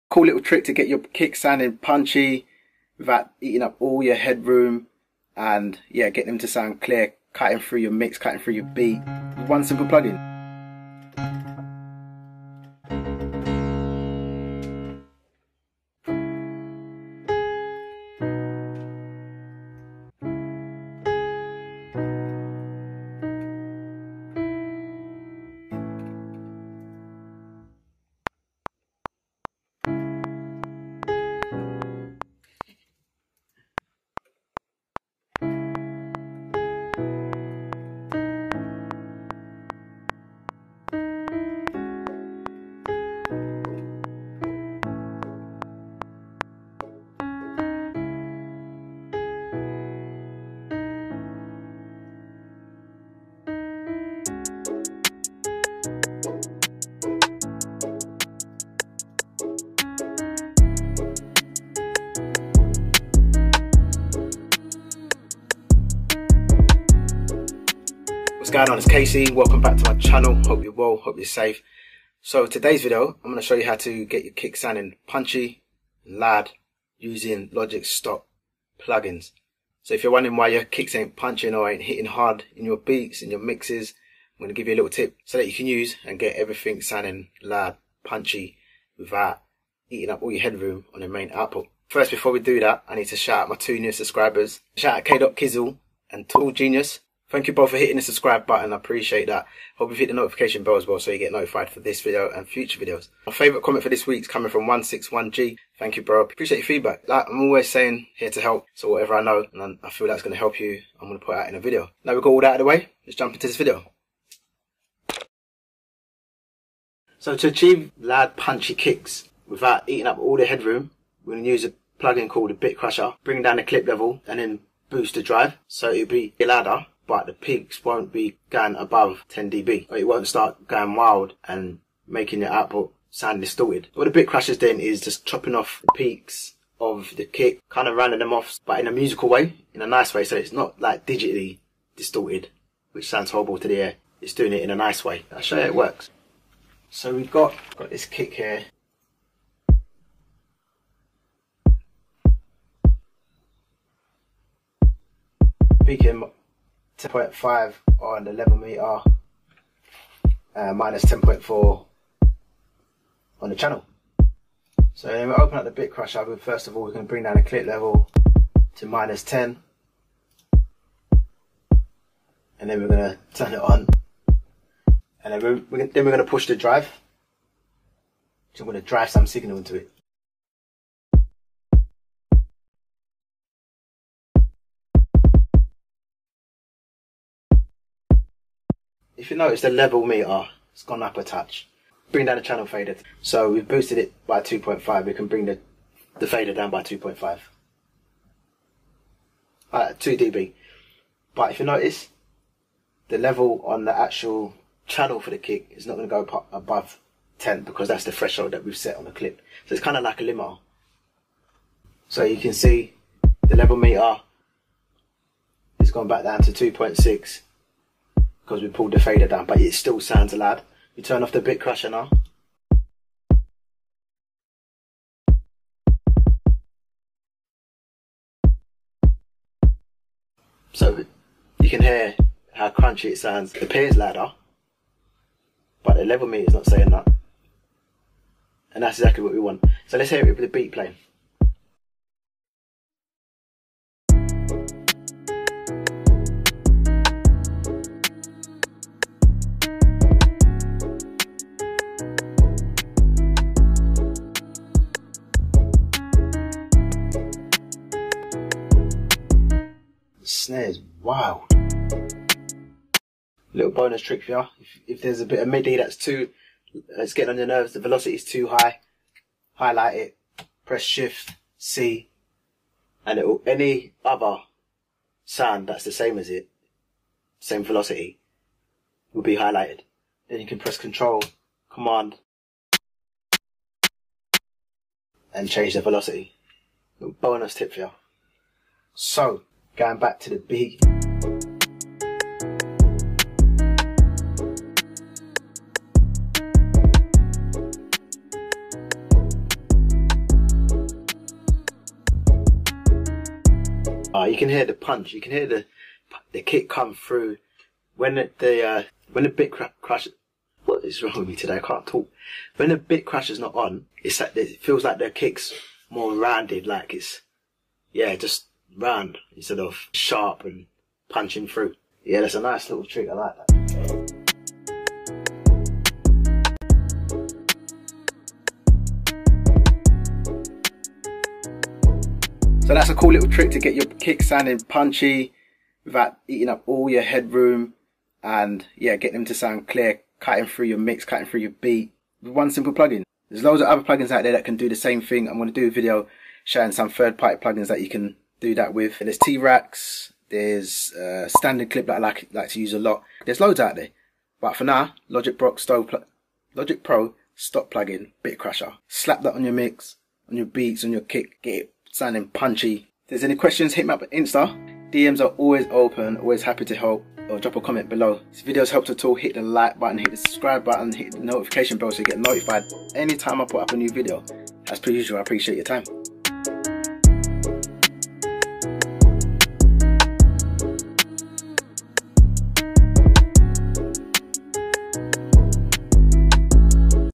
How To Make Kicks HIT HARD In Logic Pro X Loud Punchy Kicks Tutorial
How to make kick hit hard beats in Logic Pro X.
Links:
shorturl.at/hKT27